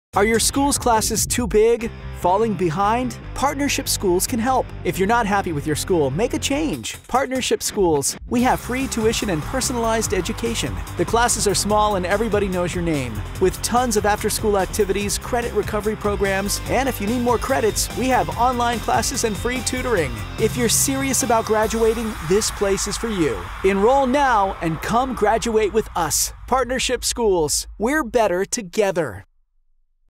Partnership Schools - Make a Change
Are Your School Classes Too Big? Falling Behind? Partnership Schools Can Help. If You're Not Happy With Your School, Make a Change. At Partnership Schools, We Have Free Tuition & Personalized Education. The Classes Are Small and Everybody Know's Your Name. With Tons Of After-School Activities, Credit Recovery Programs & If You Need More Credits, We Have Online Classes & Free Tutoring. If You're Serious About Graduating, This Place Is For You.